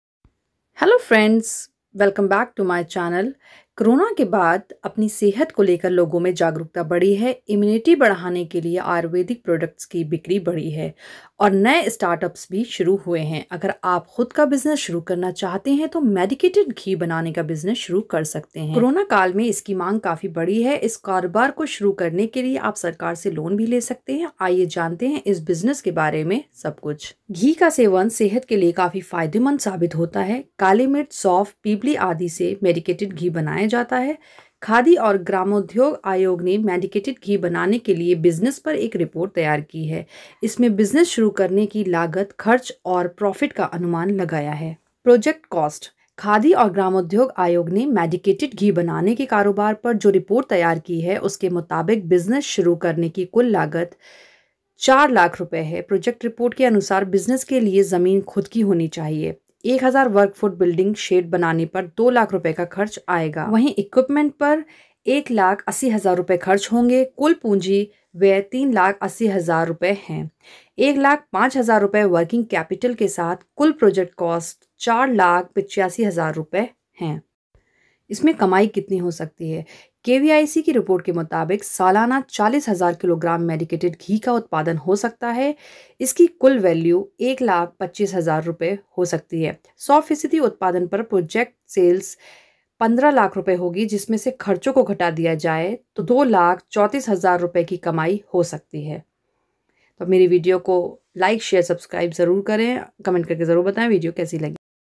Business ideas: 4.8 लाख रूपए में शुरू करे मेडीकेटेड घी बनाने का काम, हर महीने कमाए मोटा मुनाफा 🤑
Medicated ghee

Benefits of medicated ayurvadic ghee